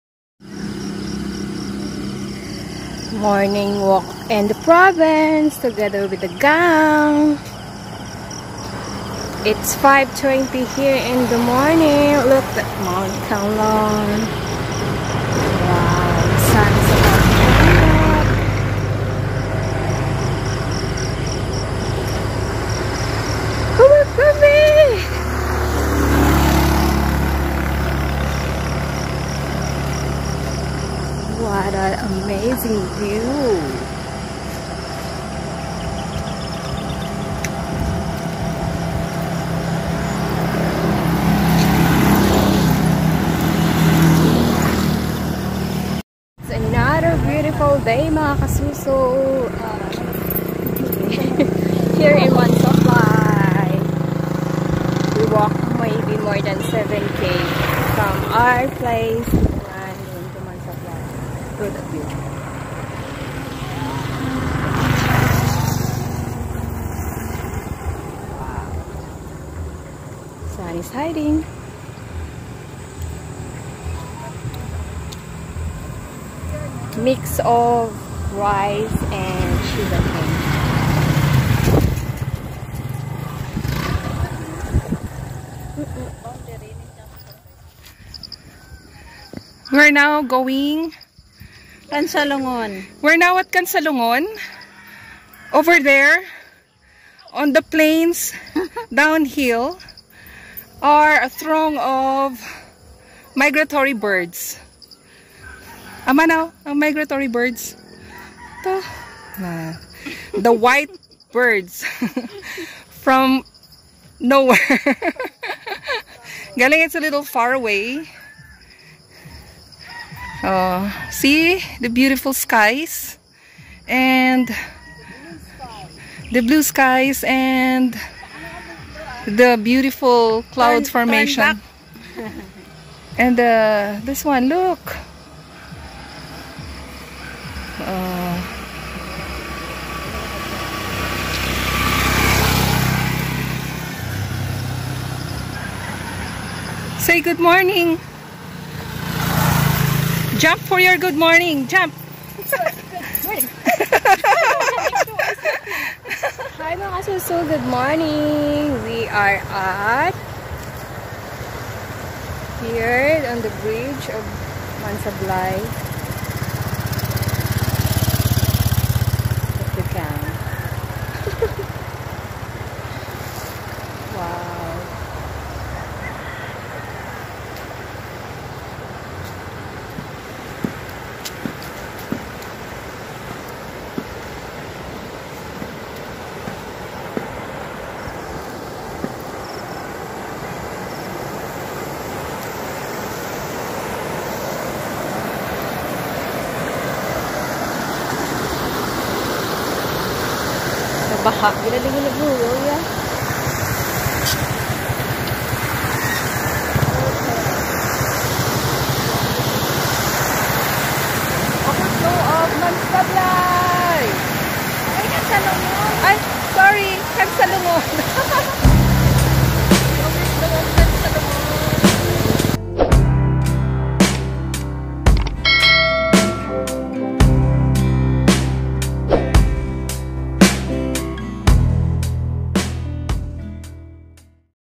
10,000 Steps Morning Walk in the Province
Morning Walk in the Province
 The 10, 000 steps.

Tinongan, Mansablay to Kansalungon Negros Occidental Philippines.